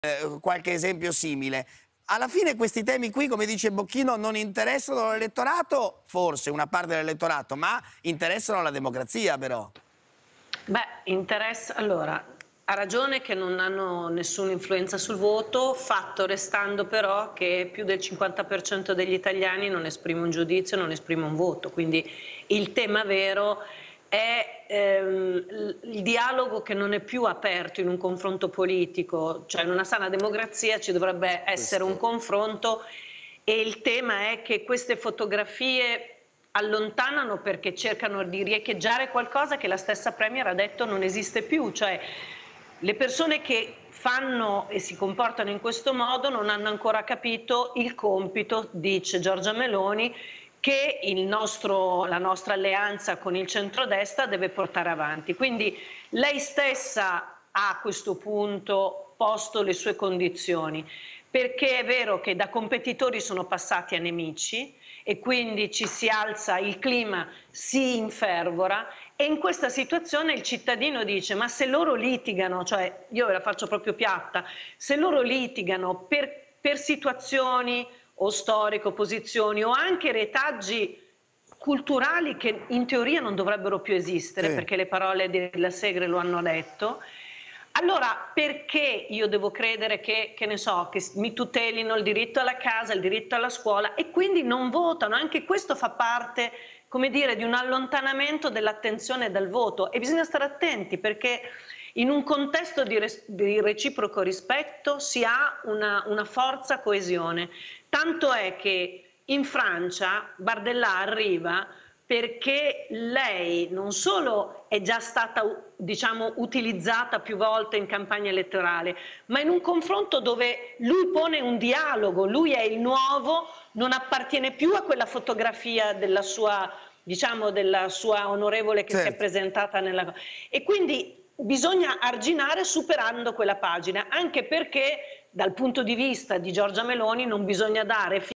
Inchiesta Fanpage, Alessandra Ghisleri: "Non ha nessuna influenza sul voto ma più del 50% ...
Alessandra Ghisleri (Euromedia Research): "Meloni ha posto le sue condizioni. Dal suo punto di vista non bisogna dare fiato alle opposizioni di parlare di temi che evidentemente la pongono in difficoltà nel momento in cui sta transando con l'Europa".